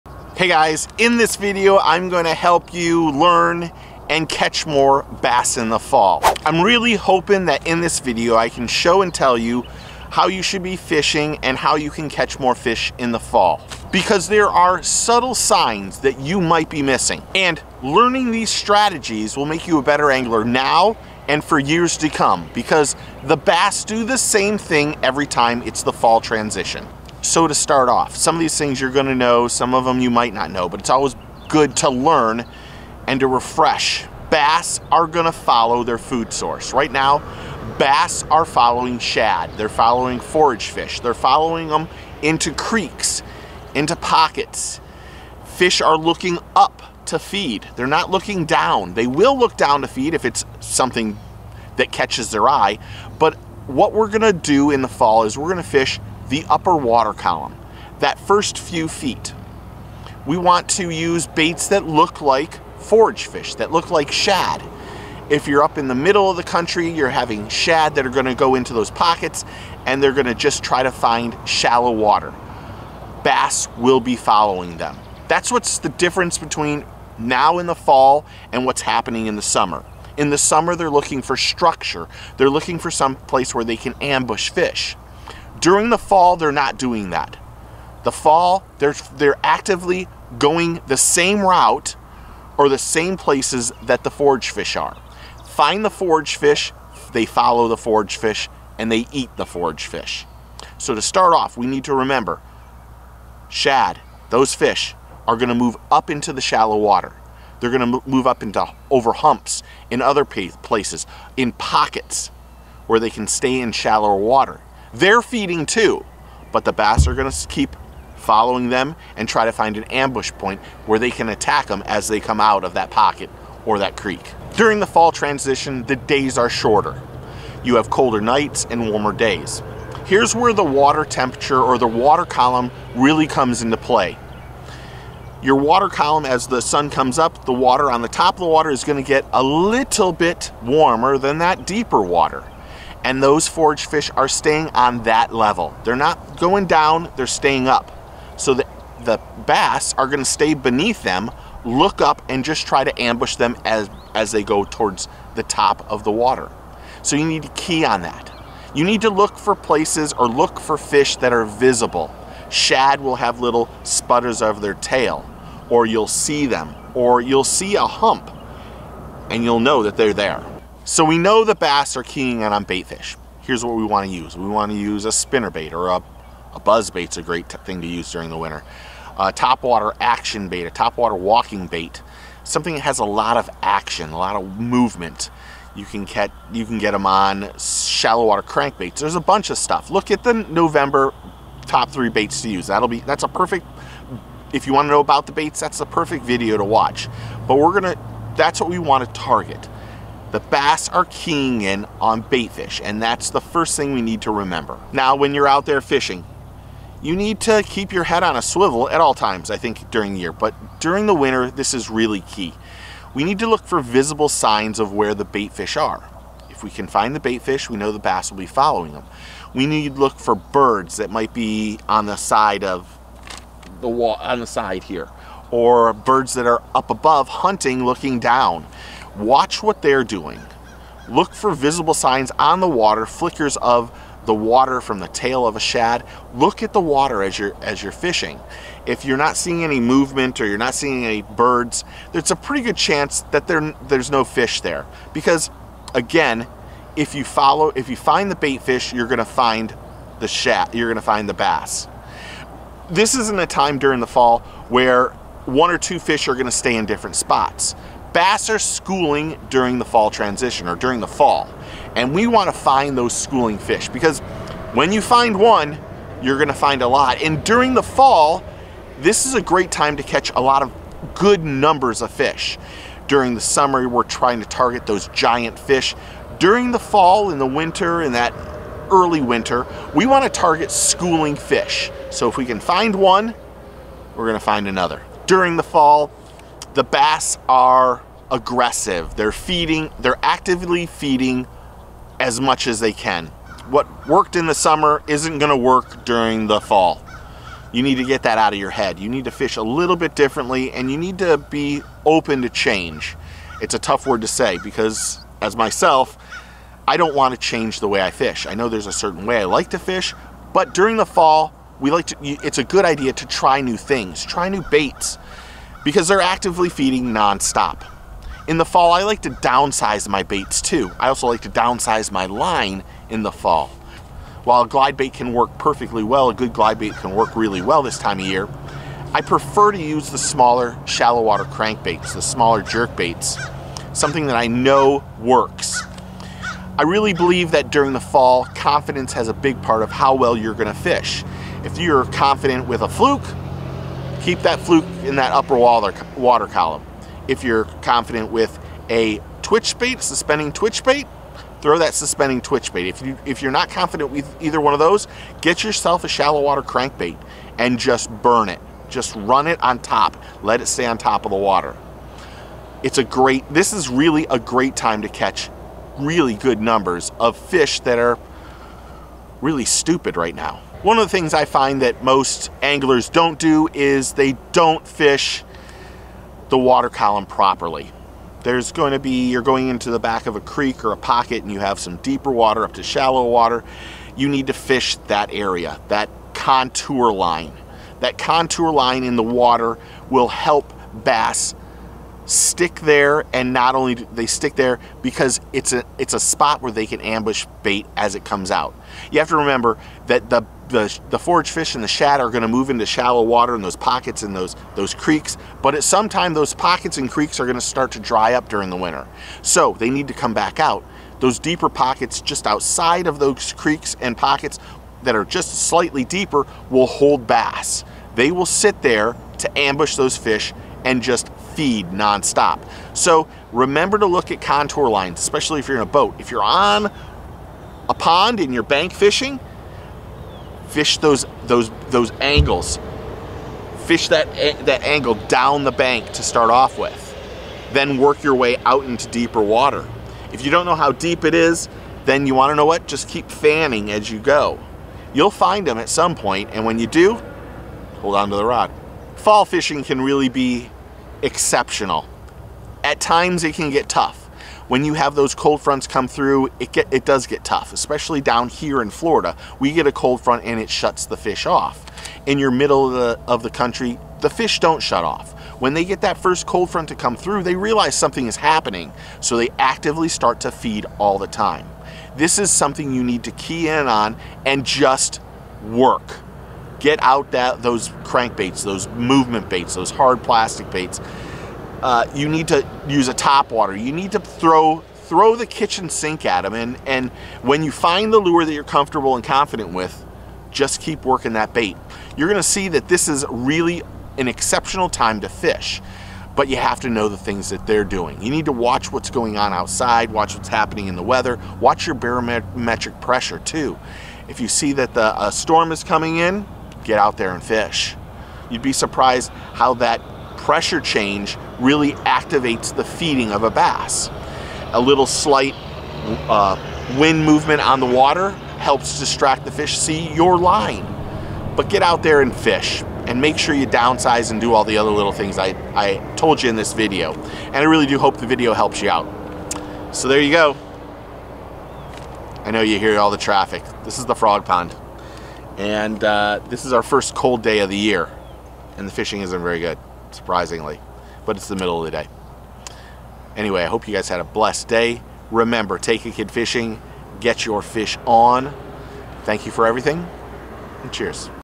Catch MORE BASS in the Fall - DOMINATE THE FALL FISHING SEASON
Want to dominate and catch more large mouth bass during the fall season on your local pond or bank fishing? Check out some of my tips, tricks and tactics for bass anglers who want to catch more bass during the fall transition. From the water column, to what you should be looking for on the water, to the migration pattern of your local bass fishery.  From the middle of the country to down south, these fall bass fishing tips and tactics will help all levels of anglers. From the beginning angler to the advanced bass fishing angler, learn from Steve and Get Ur Fish On!

Timecodes:
0:00 - Intro to Bass Fall Fishing Tips, Tricks and Techniques
0:11 - Dominate the fall bass fishing season
0:22 - Become a better angler with these strategies
0:35 - Tip 1: Bass are going to follow their food sources
0:51 - Water column to fish during the fall
1:34 - Where are the bass going and why during the fall bass fishing transition?
2:30 - Why should you be fishing the upper water column for fall bass?
3:00 - Baits to use during the fall transition for largemouth bass?
3:30 - What you should be looking for during the fall bass fishing run.
4:16 - What are large mouth bass doing during the fall?
5:01 - What time are the bass eating during the fall?
5:25 - Should you downsize your lures during the fall?
6:00 - Confidence baits...USE EM!!
6:32 - Contour lines when bass fishing
8:21 - Barometric pressure when bass fishing
9:00 - Ending, Like Comment and Subscribe!
7:36 - Fall fishing can be AWESOME